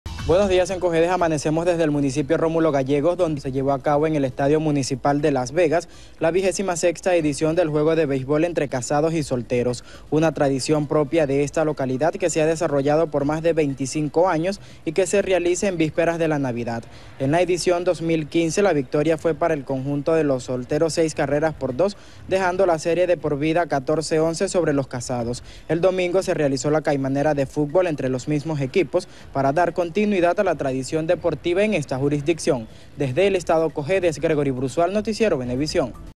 Realizan vigésima sexta edición de juegos de softbol entre solteros y casados en Cojedes
Suscríbete En Cojedes amanecemos desde el municipio Rómulo Gallegos donde este sábado se llevó a cabo en el Estadio Municipal de Las Vegas, la vigésima sexta edición del juego de softbol entre casados y solteros, una tradición propia de esta localidad que se ha desarrollado por más de 25 años.